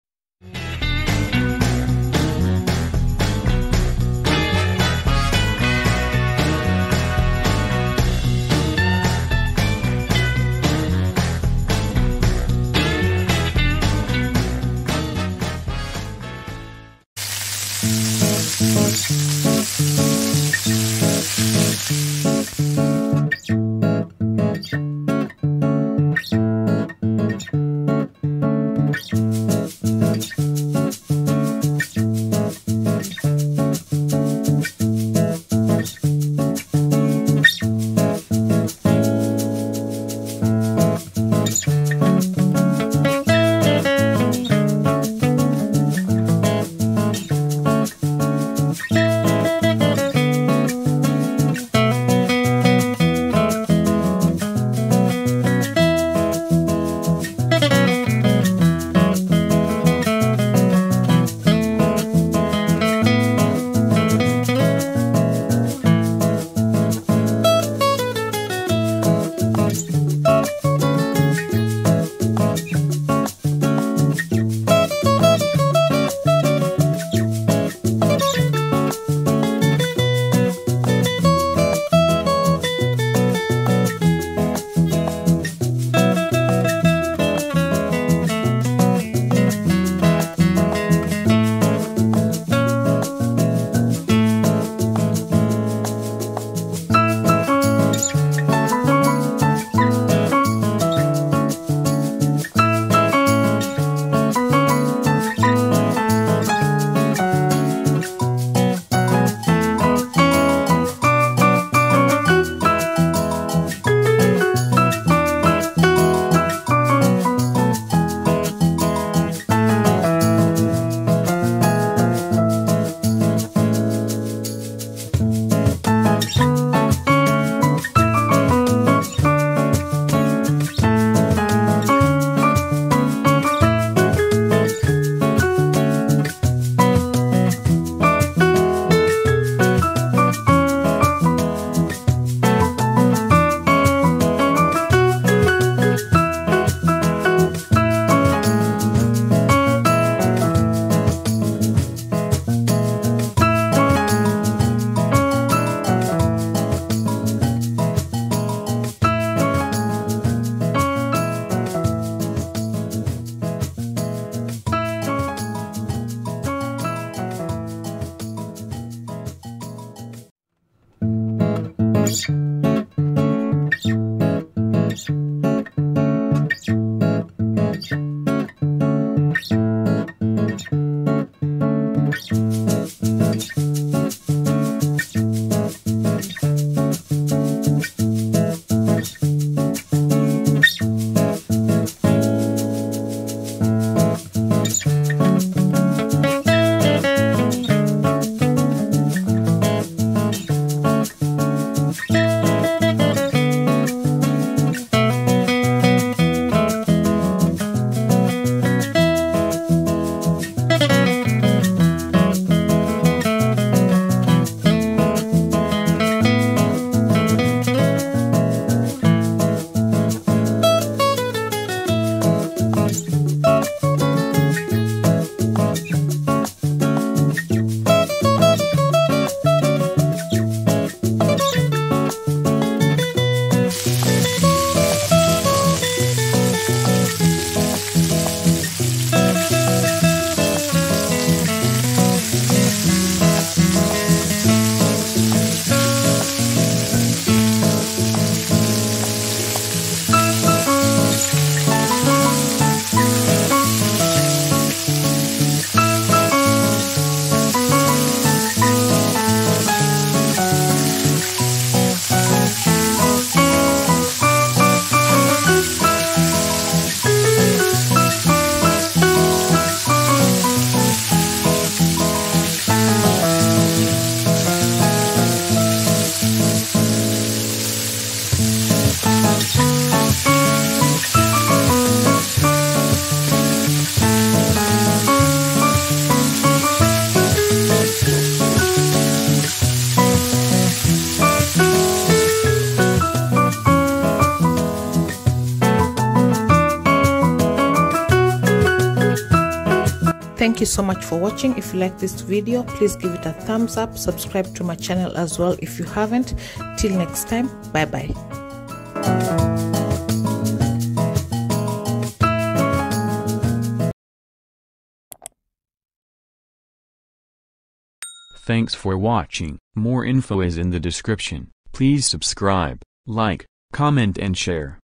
Easy Pan Roasted Chicken Recipe Pan Kuku Choma
Pan Roasted Chicken / Pan Kuku Choma is a favorite and one I enjoy making especially when i miss Kenyan Kuku Choma. Hope you enjoy this Recipe.